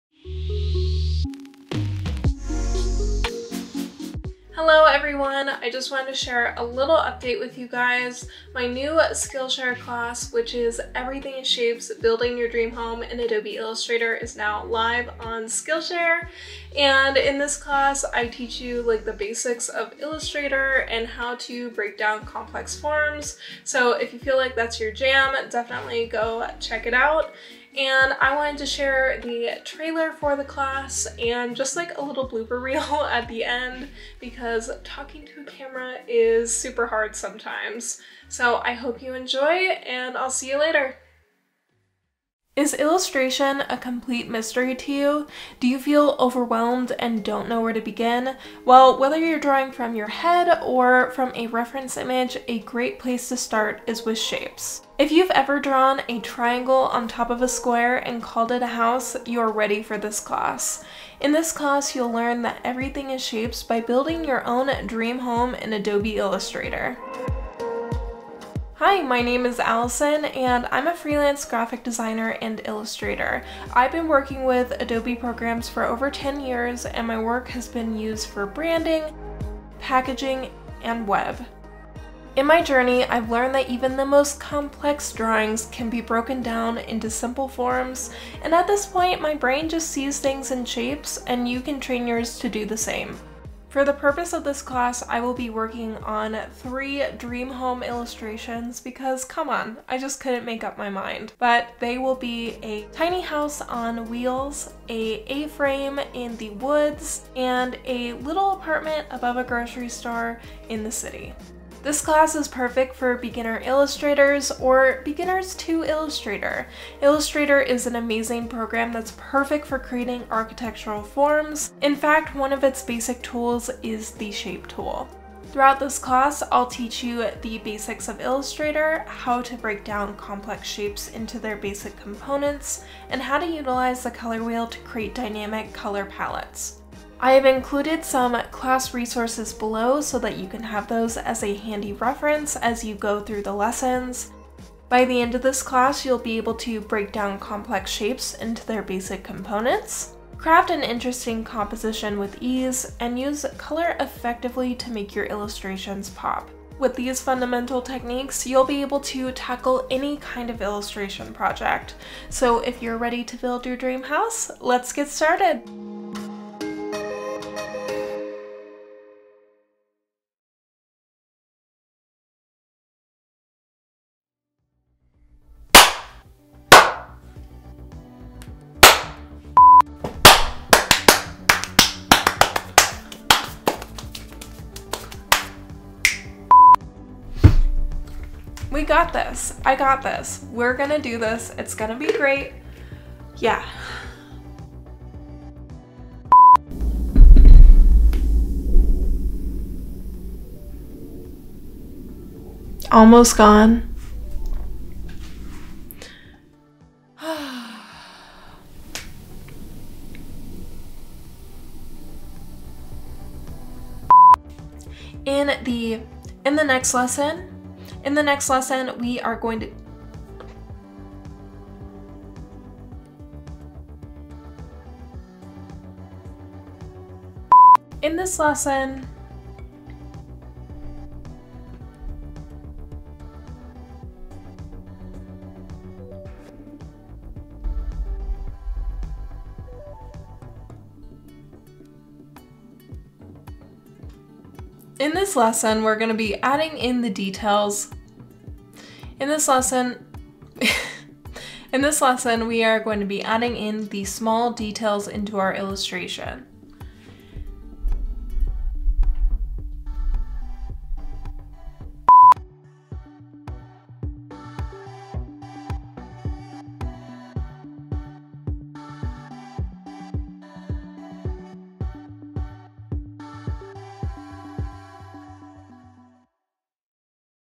An Announcement [ + blooper reel ]
Hey all! In this video, I just wanted to share the trailer for my new Skillshare class! Here is a link if you want to check it out and get 2 weeks free!
CHECK IT OUT:
alisonkoehler.design
CONNECT:
Be: @alikoeh
Footage from Adobe Fresco
Edited in Adobe Premiere Pro